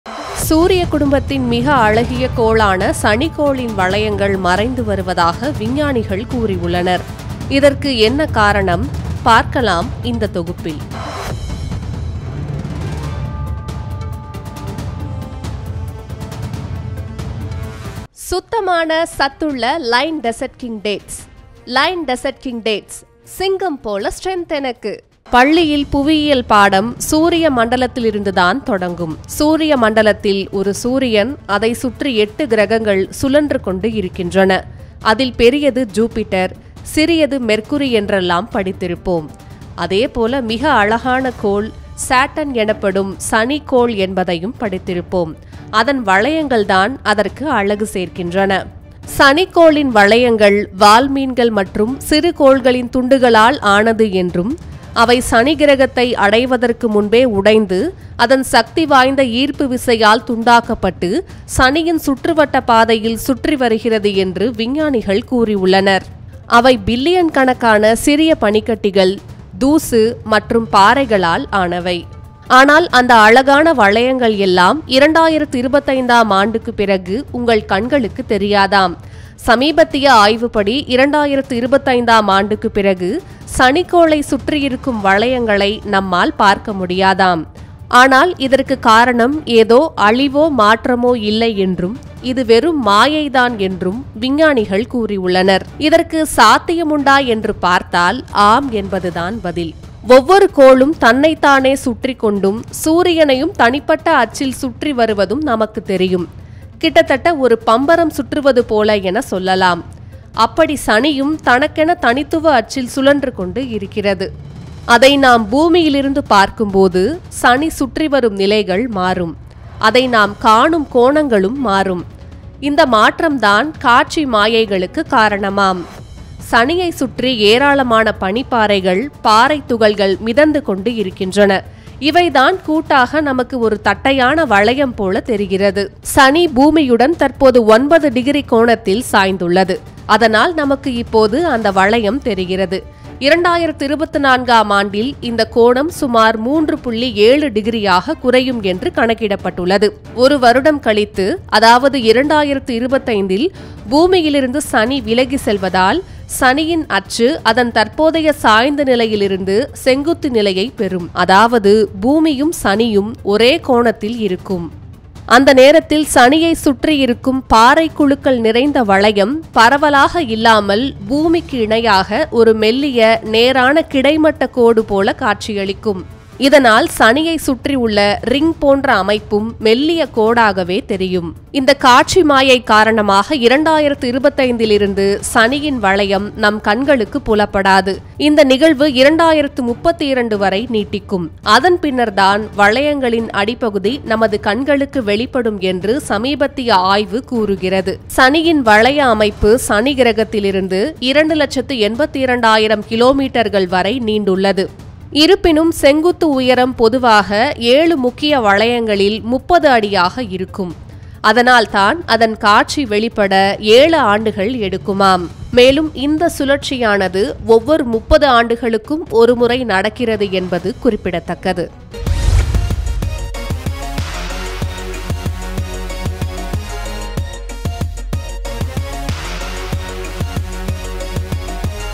சனி கிரகத்துக்கு என்ன ஆச்சு?விஞ்ஞானிகள் சொன்ன அதிர்ச்சி தகவல்! | Planet | Saturn
சூரிய குடும்பத்தின் மிக அழகிய கோளான சனிக்கோளின் வளையங்கள் மறைந்து வருவதாக விஞ்ஞானிகள் கூறியுள்ளனர் இதற்கு என்ன காரணம் பார்க்கலாம் இந்த தொகுப்பில்